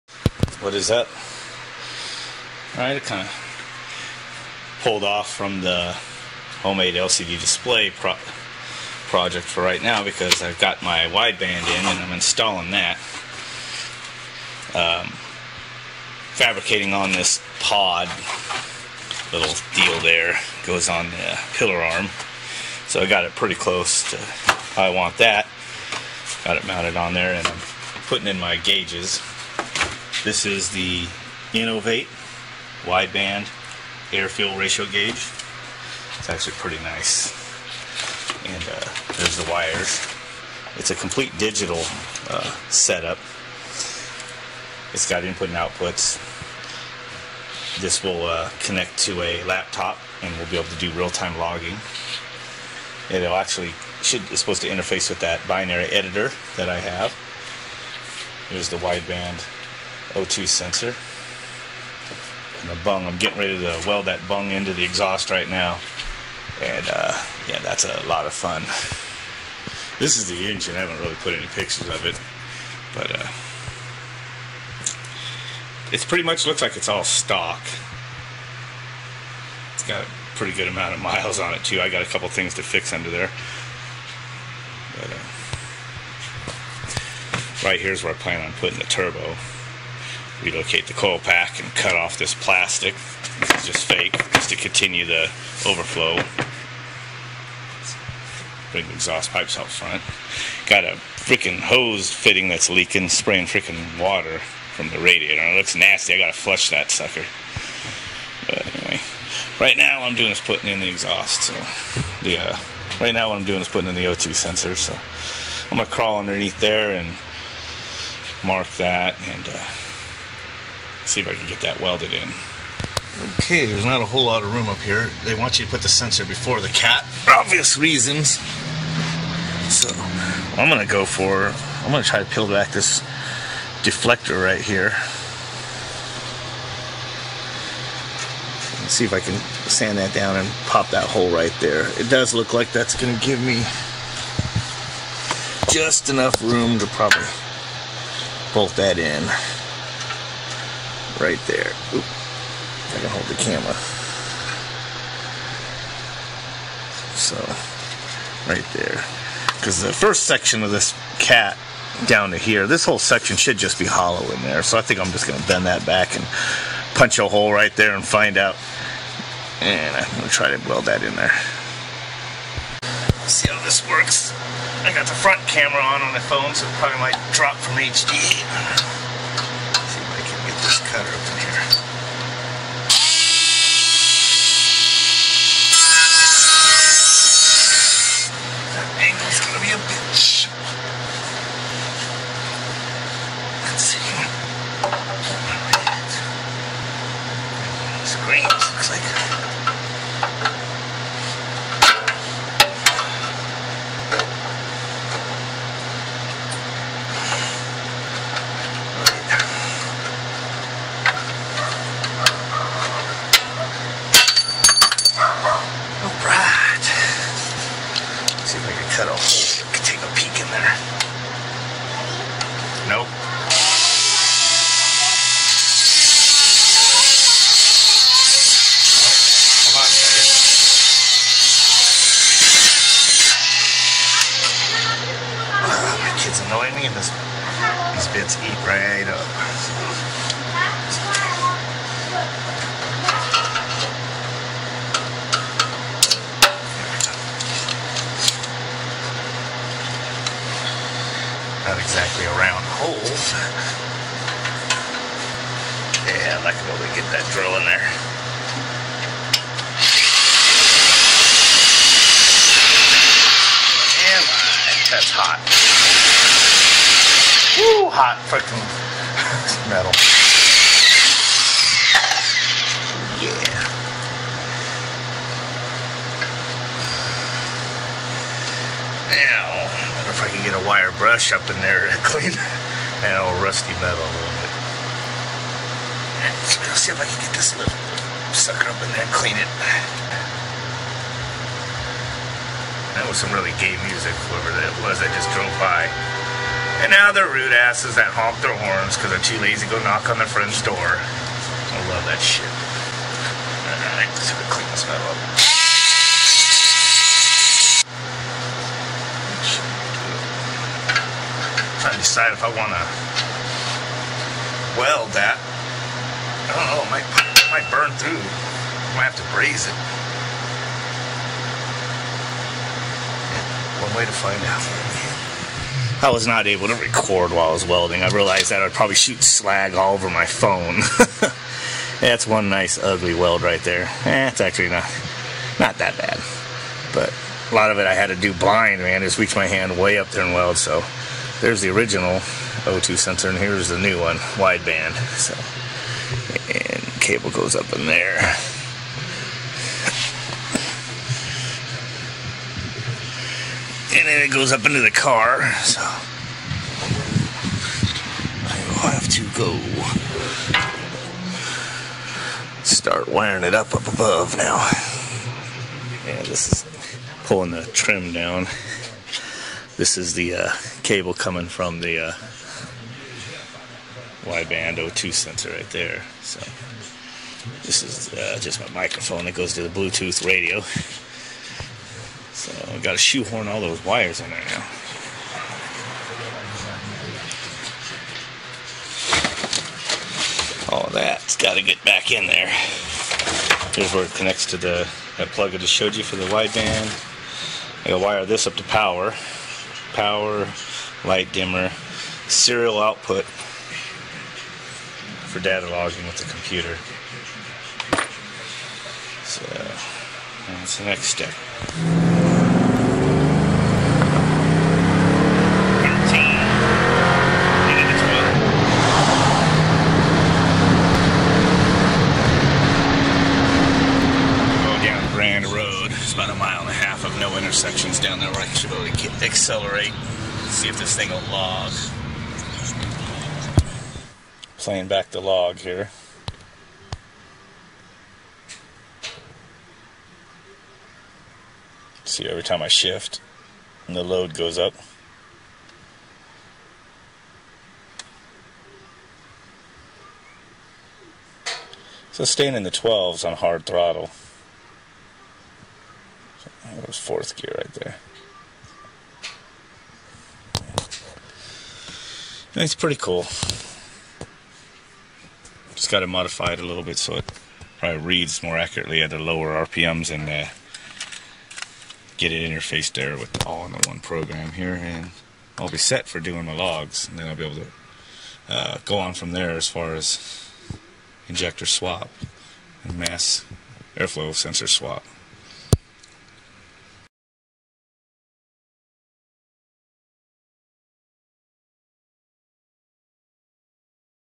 Mustang GT Project - Innovate Wideband O2 install
I had some fun getting this installed. That's why I didn't record much of it.

I started to record myself welding in the bung then realized I shouldn't for the sake of the camera (laying under the car with me using the front camera to record..

I later found out that the few seconds of welding splattered molten steel onto the glorious face of my HTC DNA phone and left a few scars. :-(

Anyway. I got the Innovate MTX installed and the interface to the laptop to log the readings.
I do need to modify the sensor bung I think. The tip of the O2 barely extends past the bung they shipped and I wonder if that is why it don't read properly until I get into the RPM's.

I also need to contact the author of Binary Editor to modify my software license to interface with the innovate wideband so I can log that in the main tuner software.